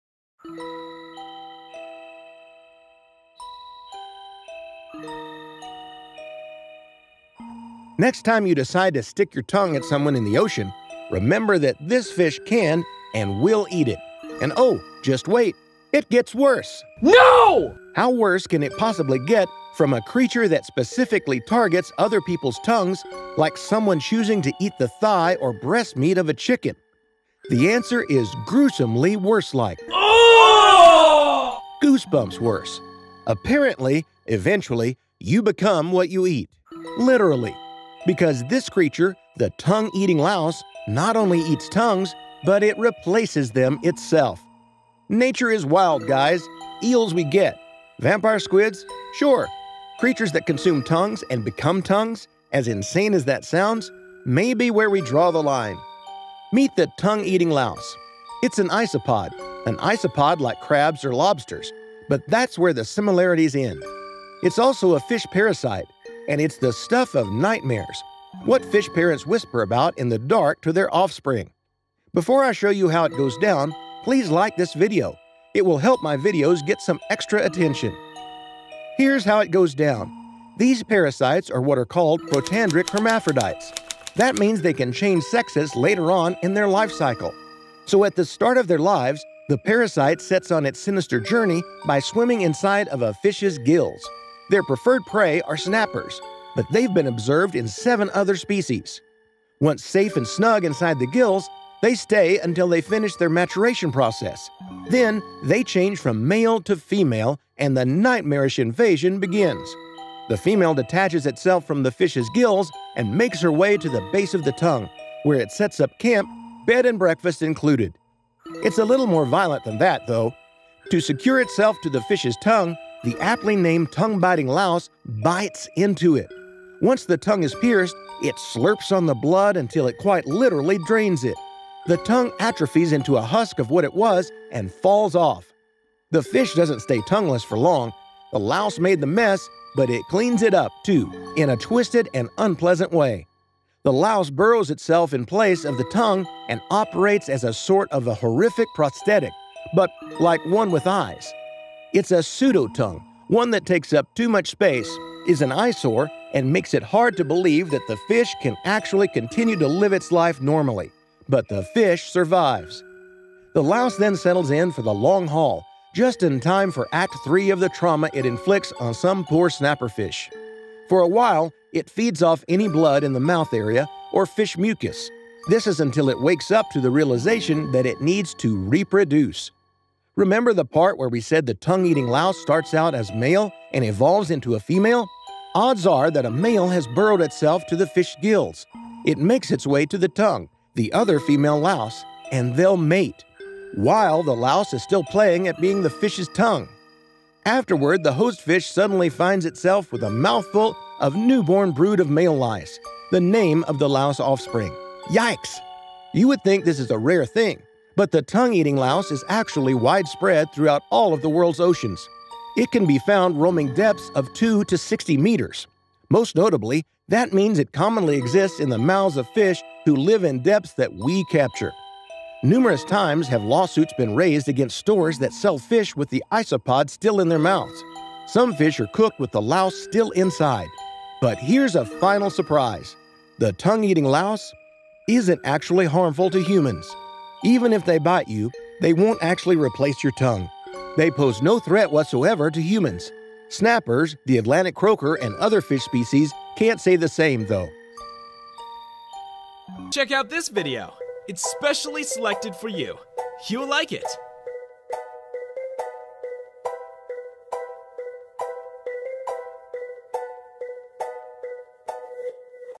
This Creature Will EAT YOUR TONGUE And Replace It!
This alien like scary creature will eat your tongue and replace it!

Every 2 weeks a new post with awesome animals!